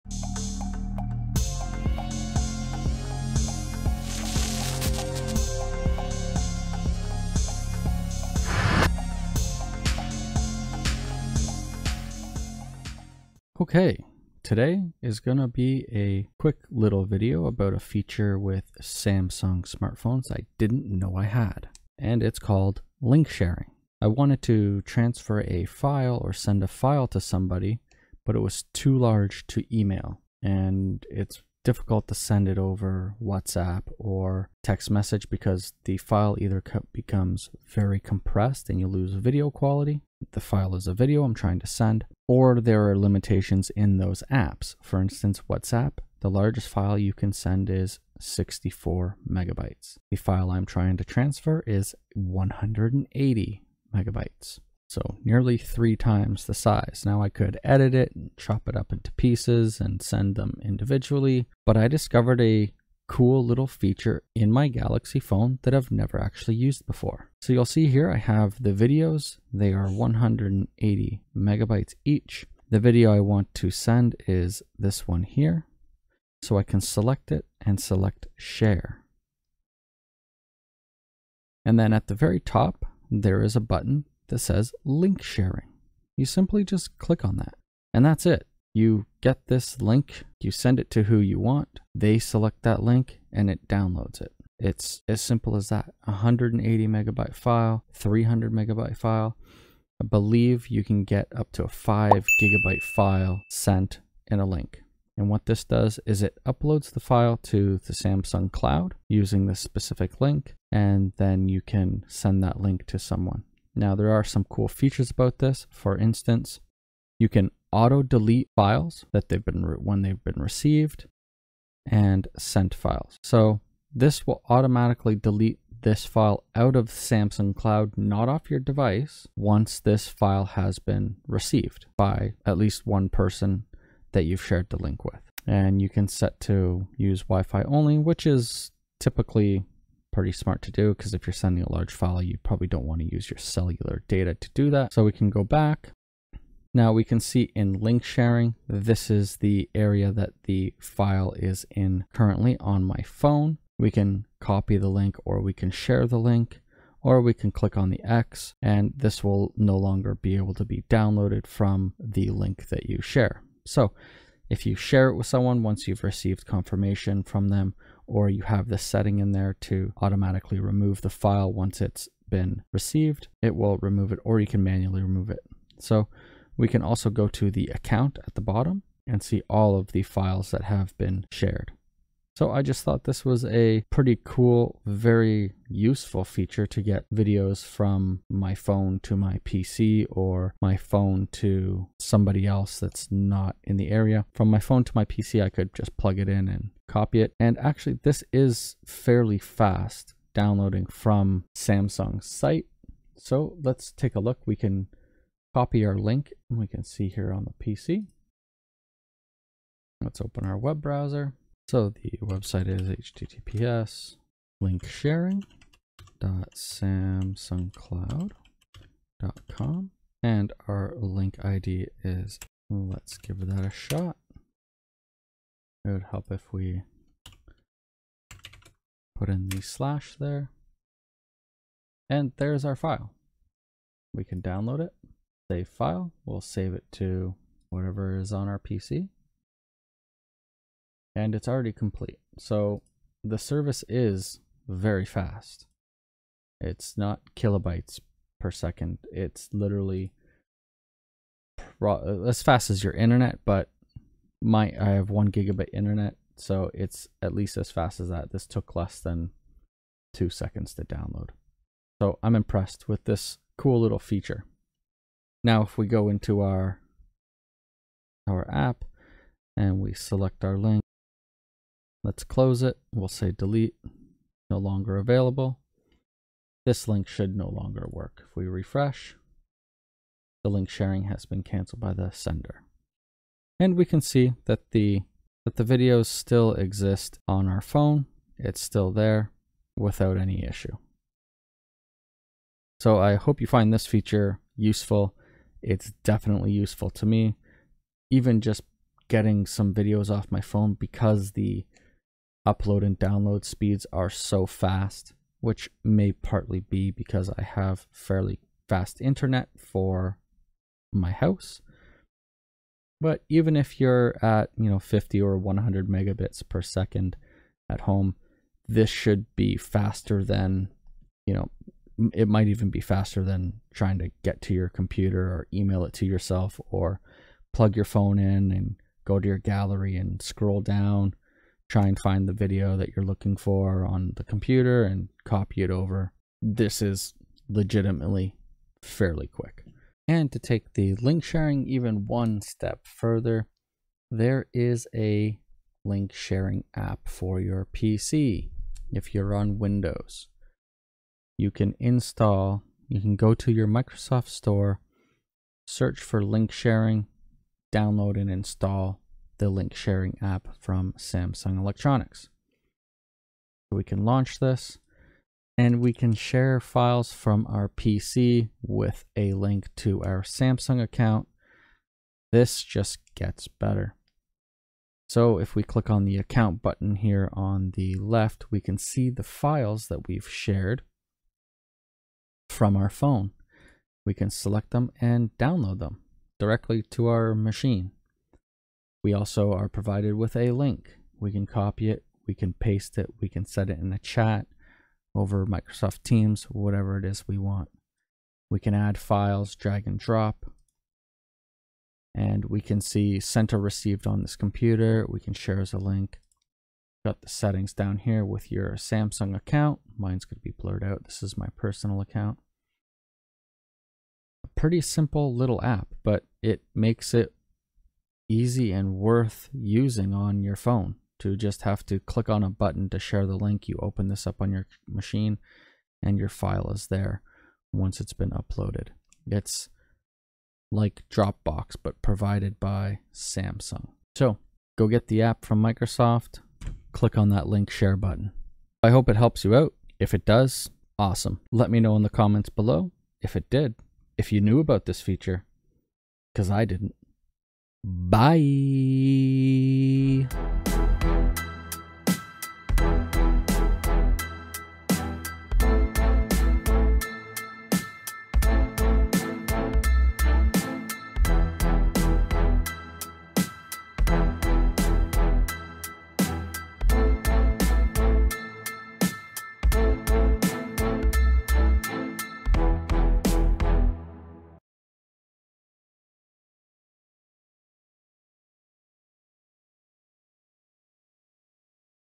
Easiest Way To Send Large Files To Anyone! Samsung Link Sharing
Link Sharing has changed to QUICK SHARE
How to move LARGE files from you Samsung phone to ANYONE
Easy and simple, 1 Click to share the link!